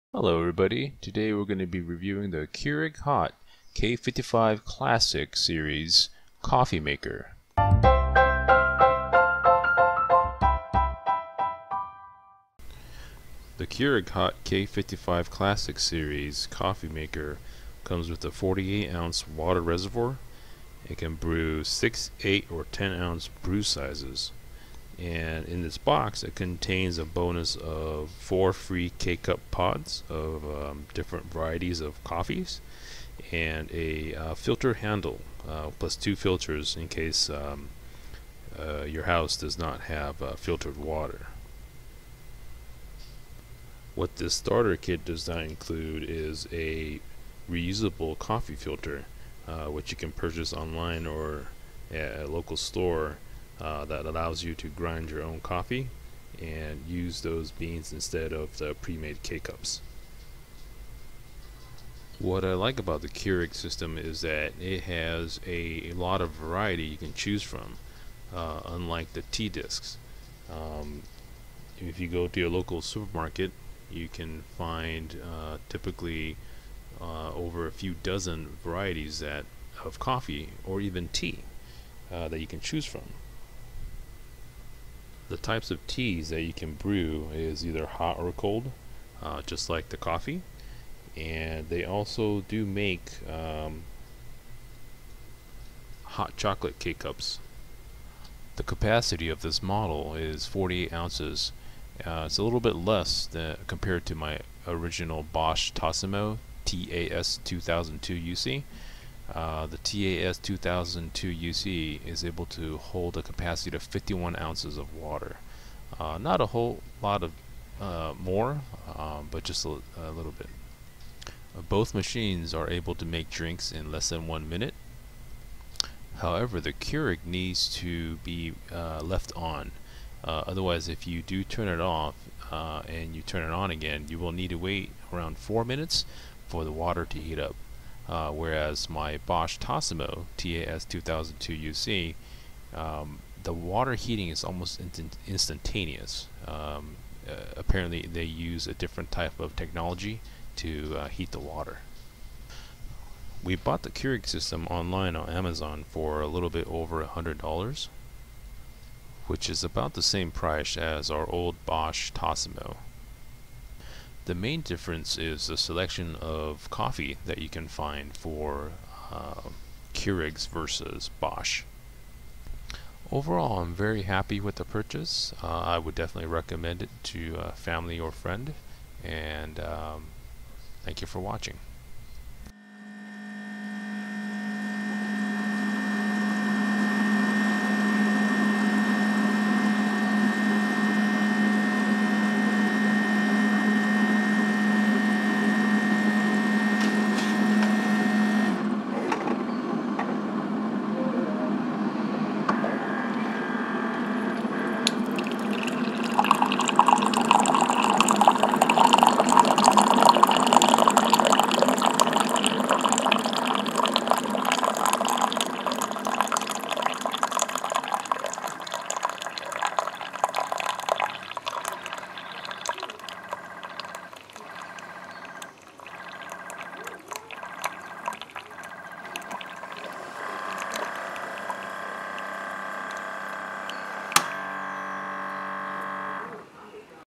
Keurig K55 Coffee Maker Review
Unpacking and review of the Keurig HOT K55 class Keurig brewing single serve coffee maker.